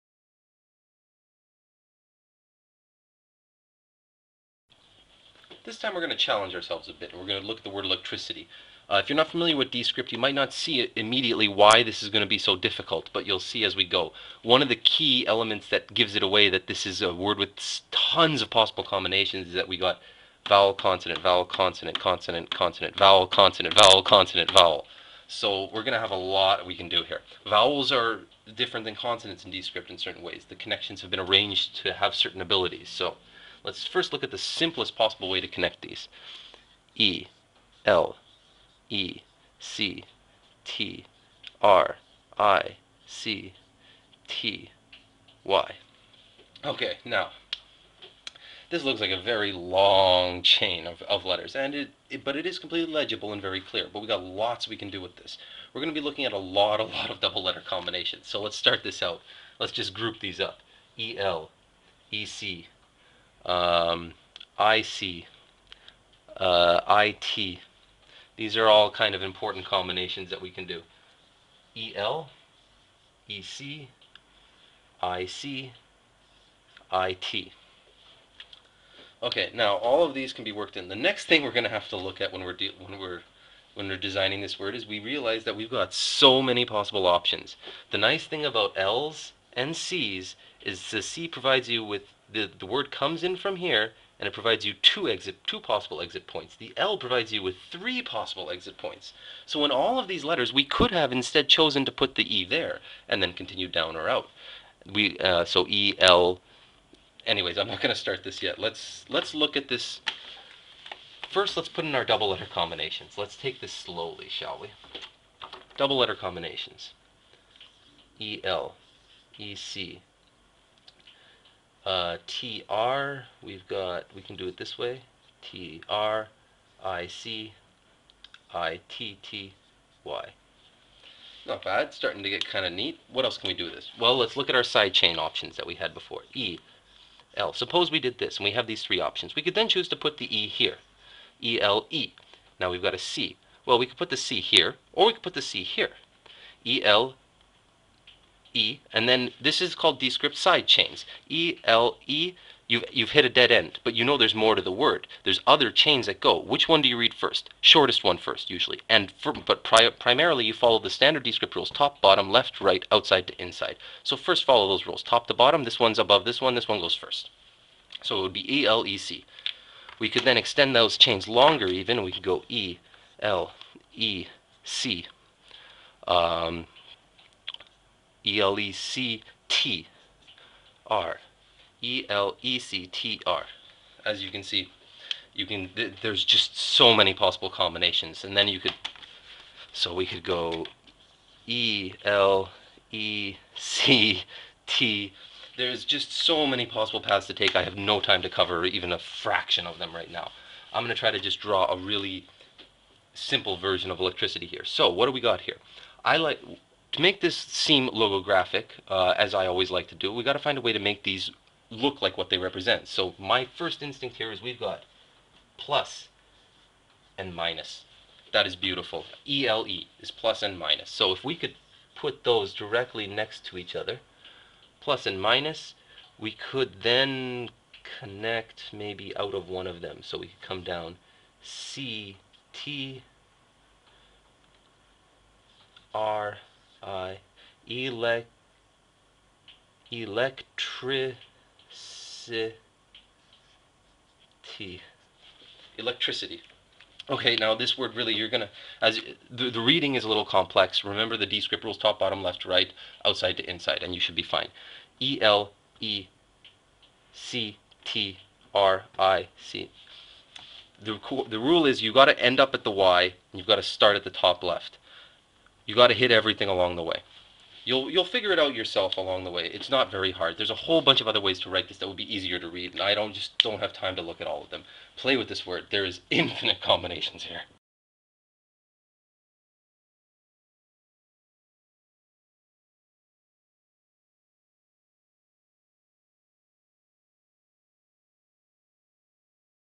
Electricity - Dscript design videos 2012 - Text art
Lets look at the word electricity. this word is VERY flexible. it allow for so many possible combinations taht we can barefly scratch the surface in this long lesson. more info on dscript at dscript.ca